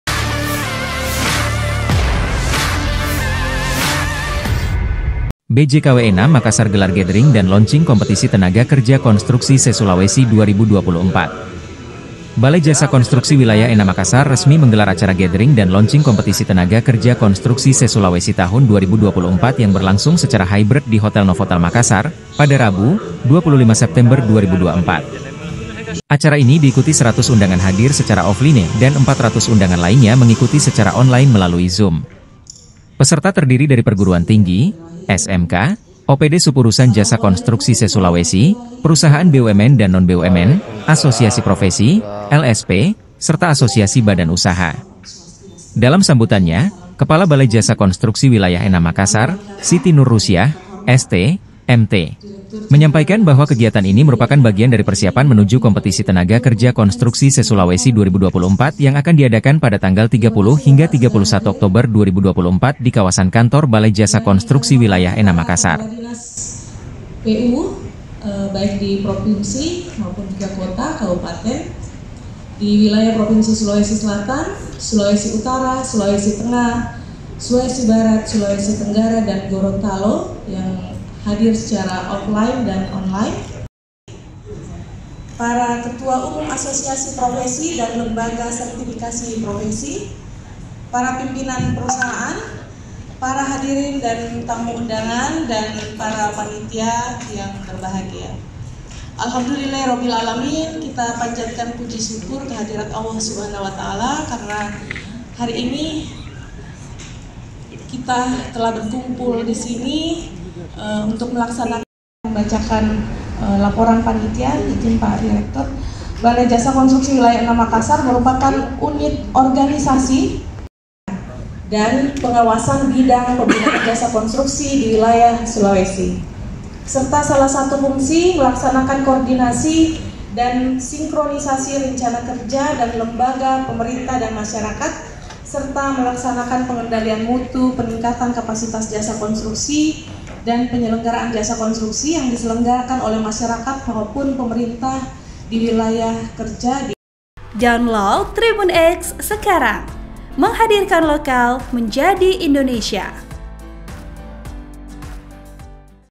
BJKW VI Makassar Gelar Gathering dan Launching Kompetisi Tenaga Kerja Konstruksi Se-Sulawesi 2024
TRIBUN-MAKASSAR - Balai Jasa Konstruksi Wilayah VI Makassar resmi menggelar acara Gathering dan Launching Kompetisi Tenaga Kerja Konstruksi Se-Sulawesi Tahun 2024 yang berlangsung secara hybrid di Hotel Novotel Makassar,  pada Rabu, 25 September 2024.

Acara ini diikuti 100 undangan hadir secara offline dan 400 undangan lainnya mengikuti secara online melalui Zoom. Peserta terdiri dari perguruan tinggi, SMK, OPD sub-urusan jasa konstruksi se-Sulawesi, perusahaan BUMN dan non-BUMN, asosiasi profesi, LSP, serta asosiasi badan usaha.

Dalam sambutannya, Kepala Balai Jasa Konstruksi Wilayah VI Makassar, Siti Nurrusiah, ST., MT., menyampaikan bahwa kegiatan ini merupakan bagian dari persiapan menuju Kompetisi Tenaga Kerja Konstruksi Se-Sulawesi 2024 yang akan diadakan pada tanggal 30-31 Oktober 2024 di Kawasan Kantor Balai Jasa Konstruksi Wilayah VI Makassar.(*)